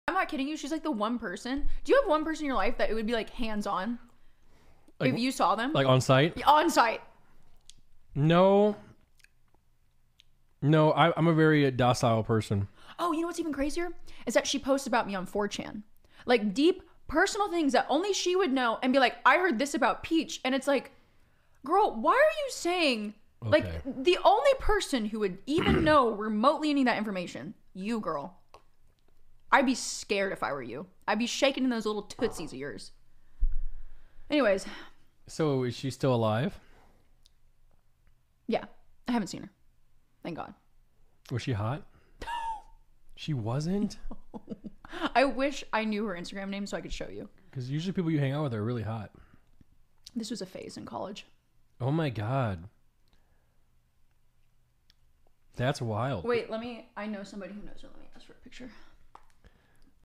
Nmplol: Peach would Put Hands on the B*tch
Nick would never hurt anyone. Peach has no problem knocking a bitch out.